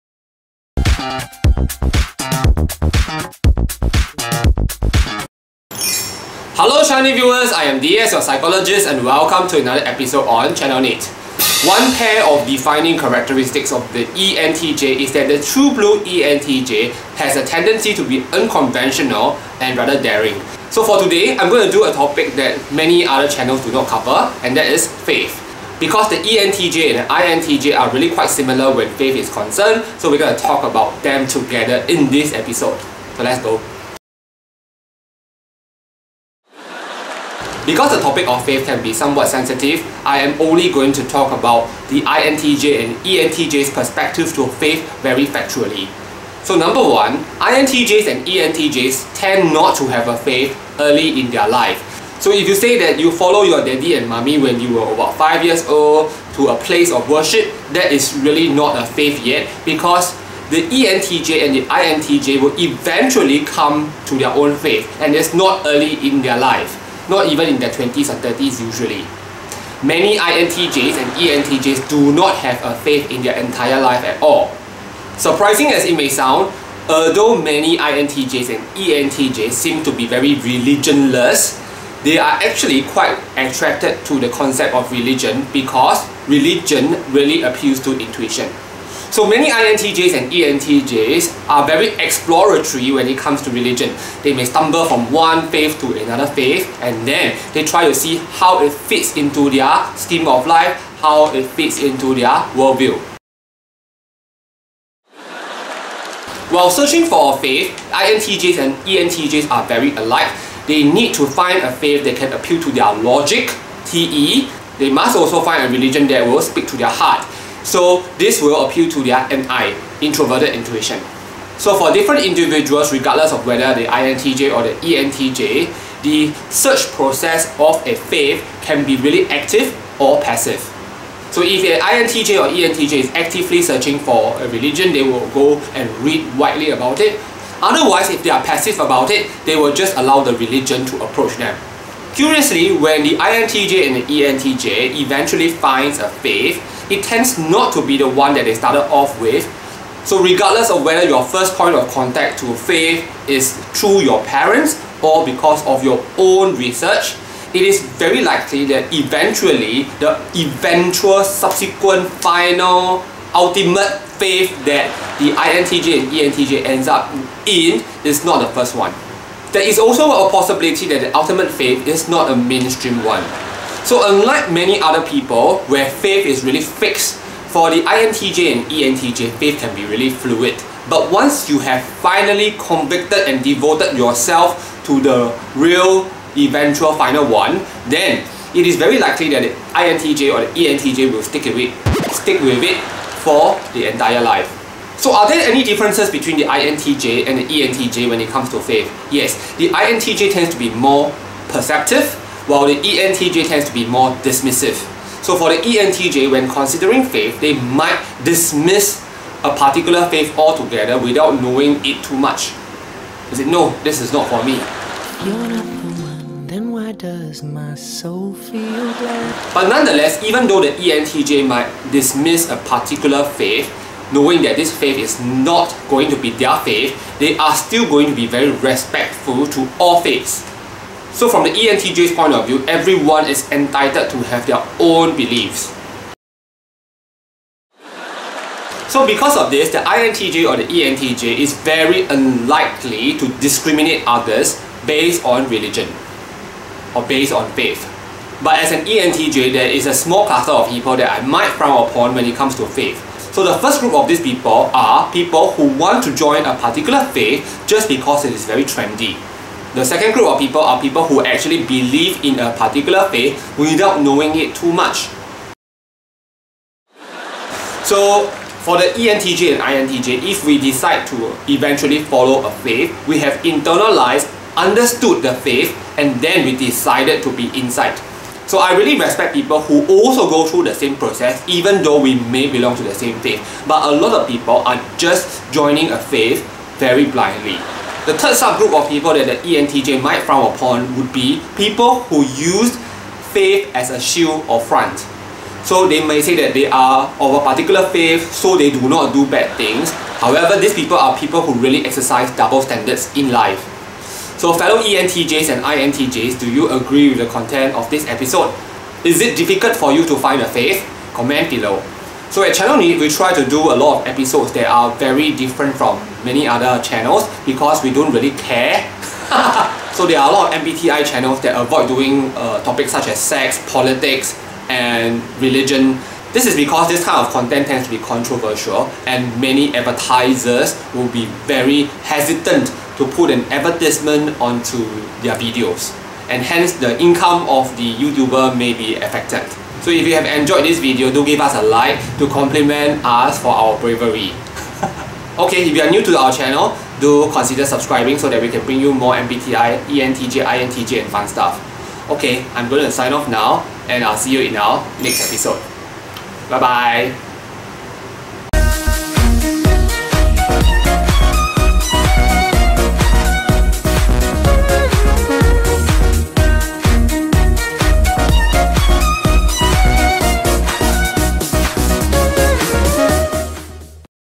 Ep 297 INTJs and ENTJs on the topic of FAITH (Purely factual)
In this episode, we challenge another topic that might be commonly avoided by other content creators. What is the INTJ and the ENTJ's attitude, and journey to having one? DS explains in this episode. (Includes what an ENTJ may dislike about others in this area)

That'll motivate us loads!